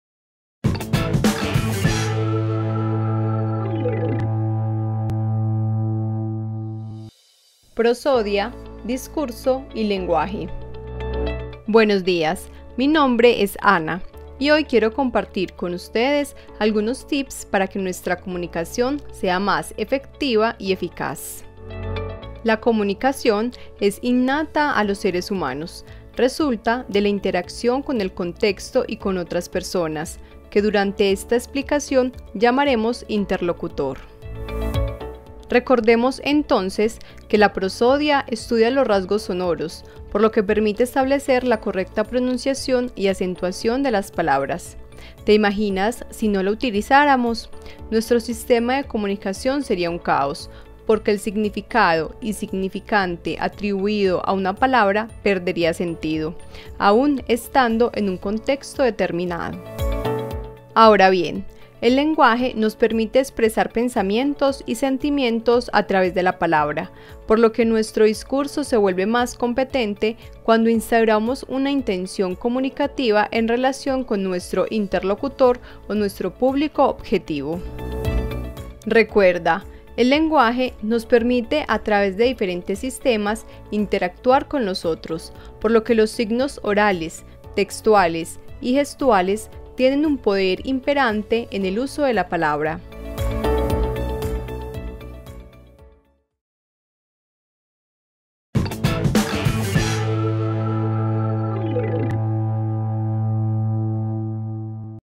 Prosodia, discurso y lenguaje
Buenos días, mi nombre es Ana y hoy quiero compartir con ustedes algunos tips para que nuestra comunicación sea más efectiva y eficaz.